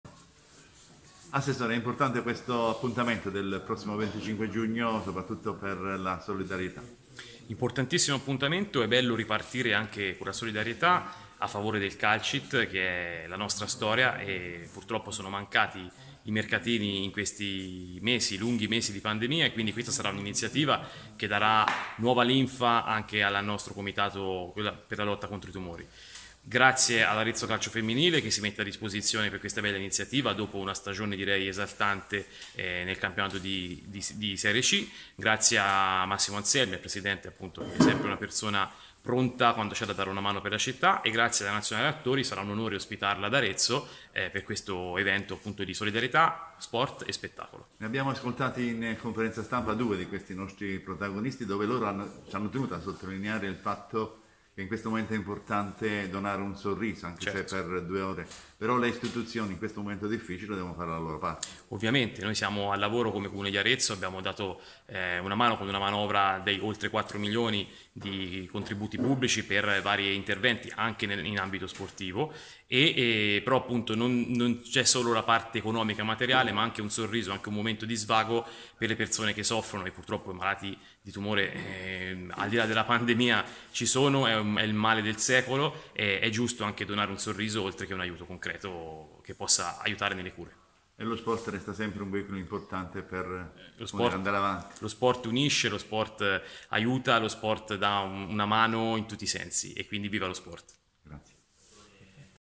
L' Assessore Federico Scapecchi a Sport a Km 0
L' Assessore Federico Scapecchi a @Sport a Km 0 presenta "La partita dei cuori uniti" Un' iniziativa all' insegna della solidarietà e della beneficenza in collaborazione con il CALCIT di Arezzo.